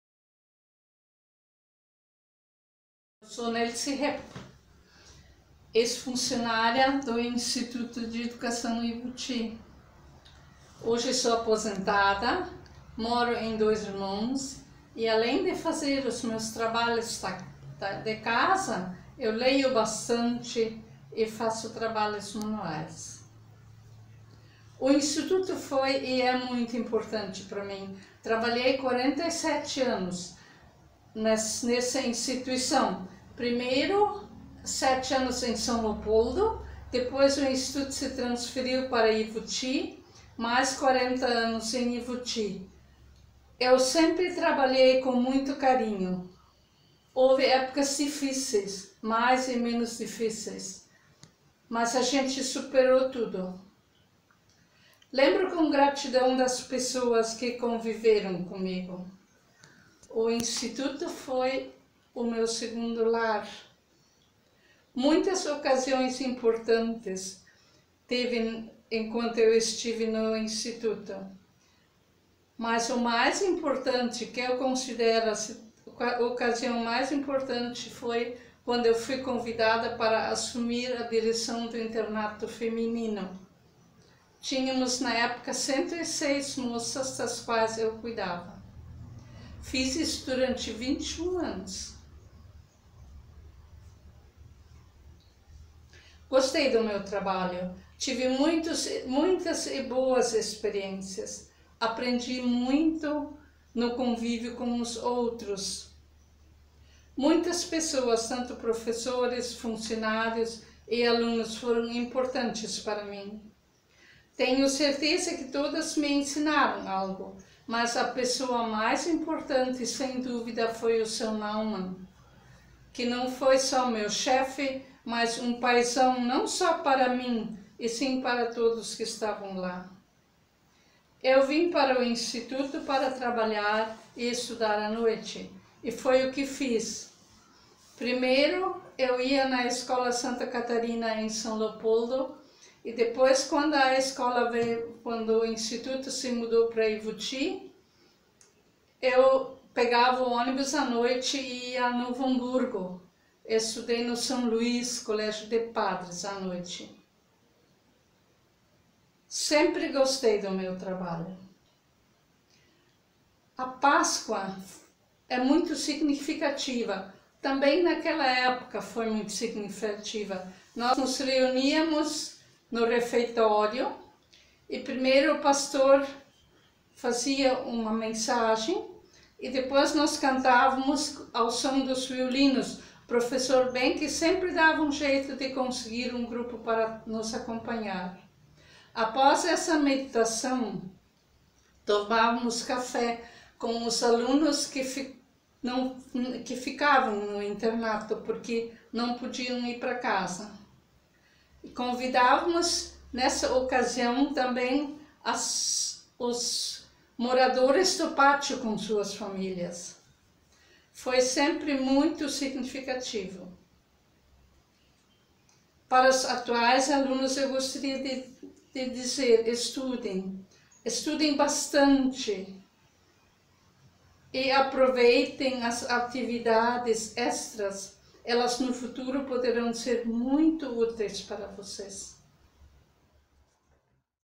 Especial 112 anos o Instituto Ivoti - Entrevista Nelsi Hepp
Depoimento coletado pelo Grêmio Estudantil Gustavo Adolfo - GEGA

Dando início a nossa sequência de entrevistas com pessoas que marcaram sua passagem no Instituto Ivoti, Nelsi Hepp nos conta um pouco de suas experiências na instituição.